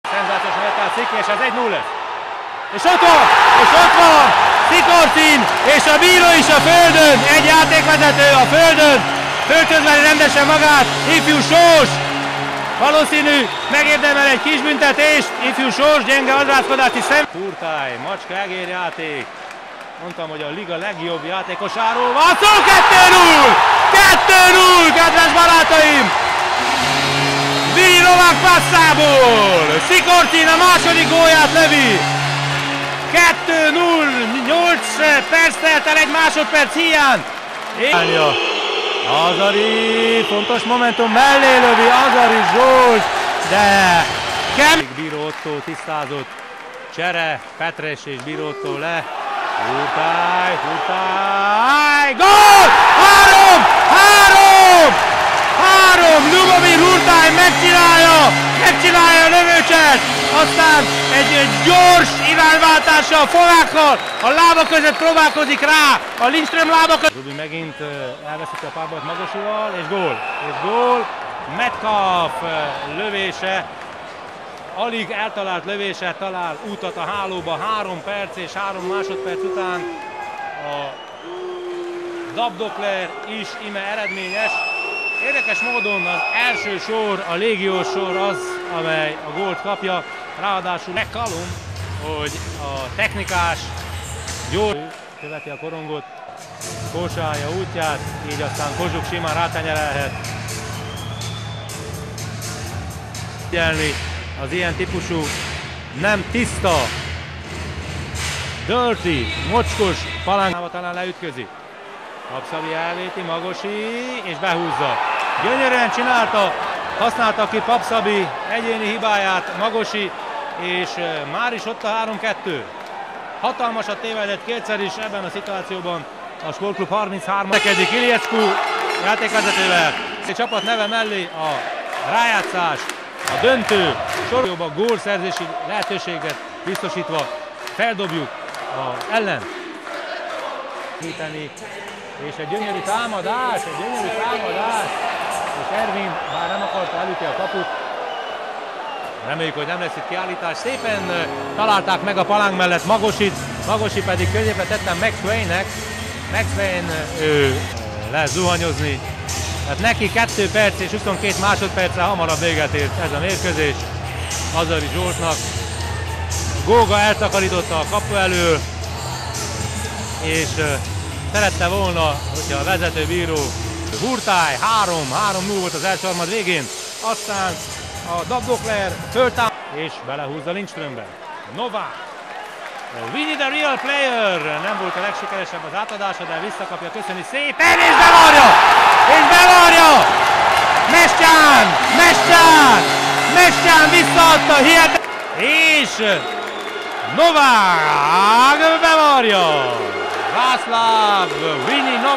HSC Csikszereda-Dab.Docler 4-2
Mol Liga-döntő, 2010/11, 1. mérkőzés, 2011. január 14.